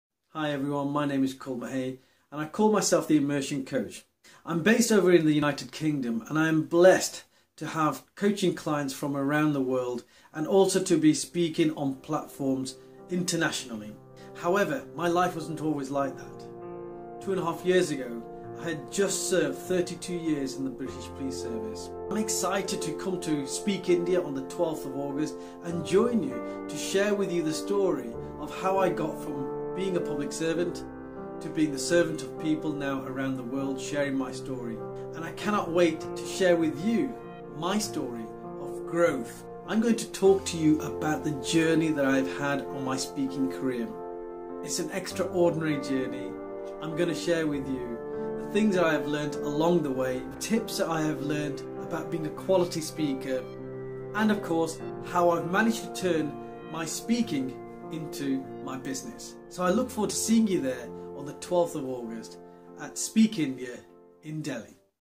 Kul Mahay - Message to SpeakIndia
My journey as a speaker and why I am looking forward to sharing it with attendees at the SpeakIndia conference in New Delhi.